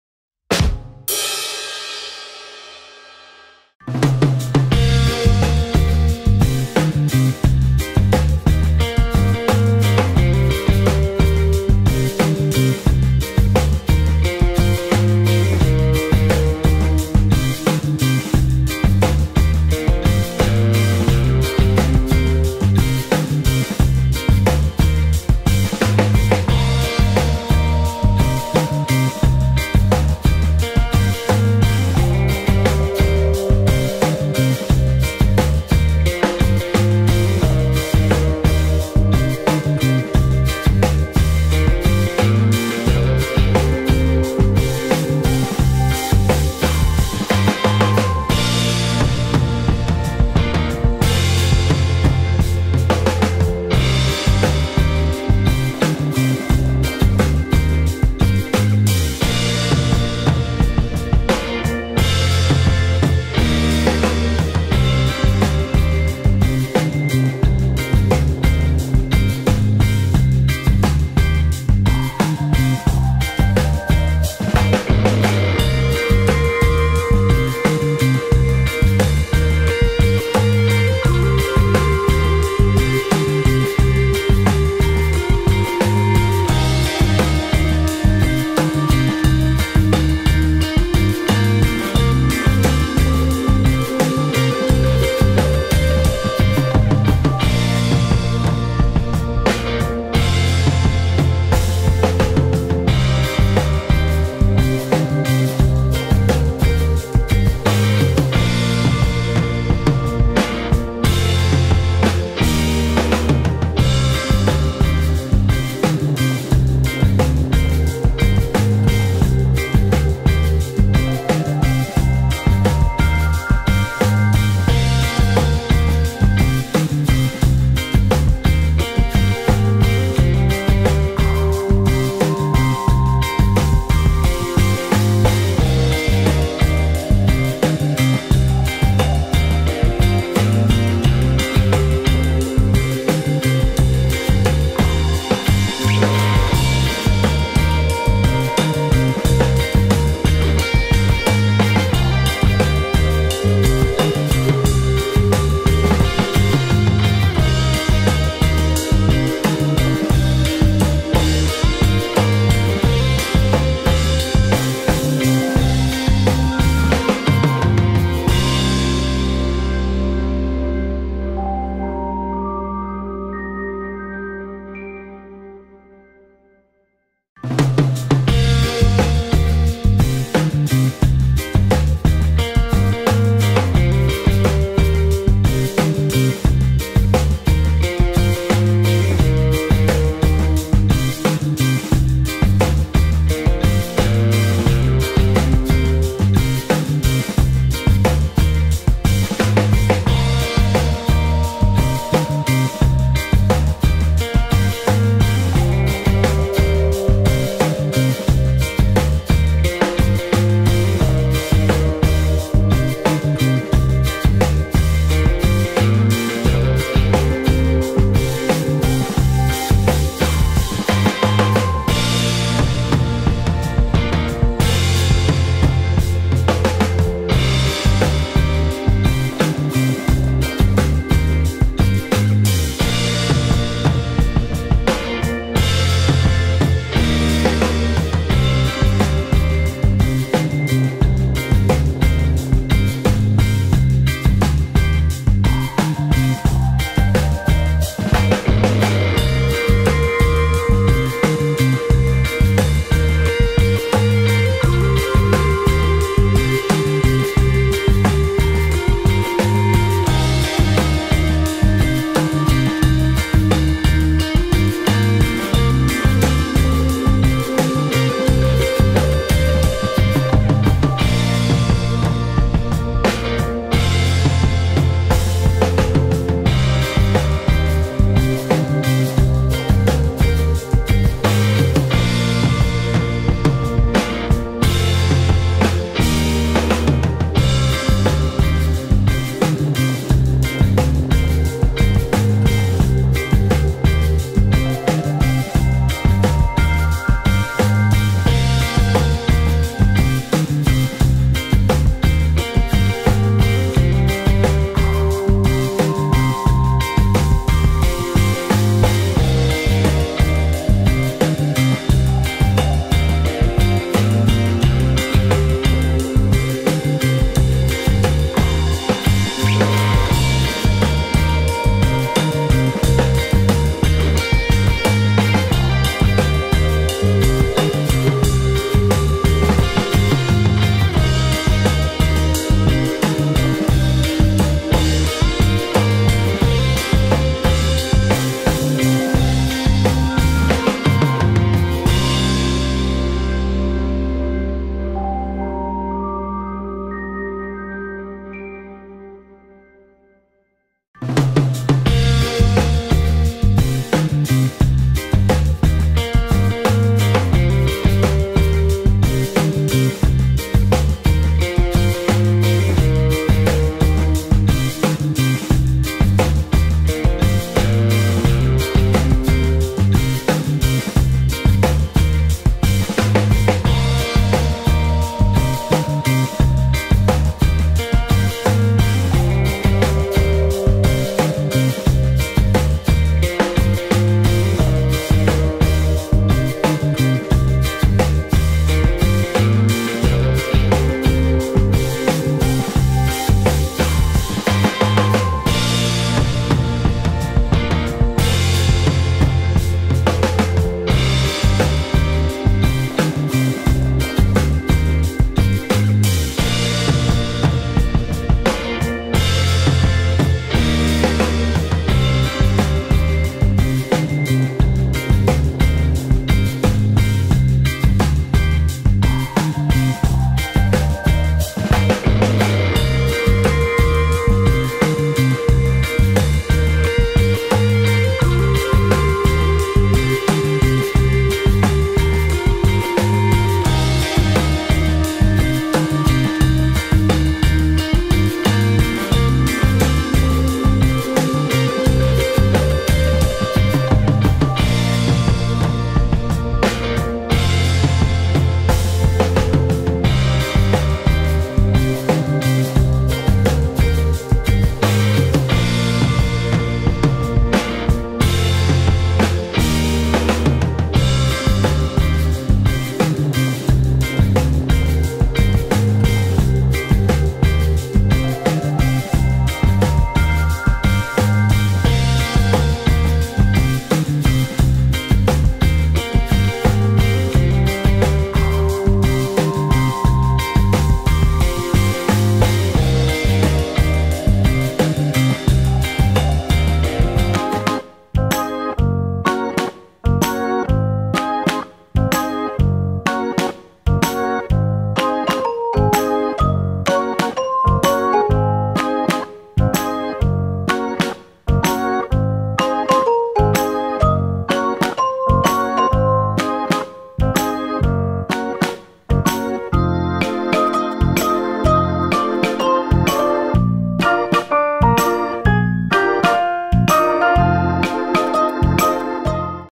Driving around Puerto Jiménez, Osa Peninsula, Costa Rica
See what Puerto Jiminez, the biggest town in the Osa Peninsula looks like.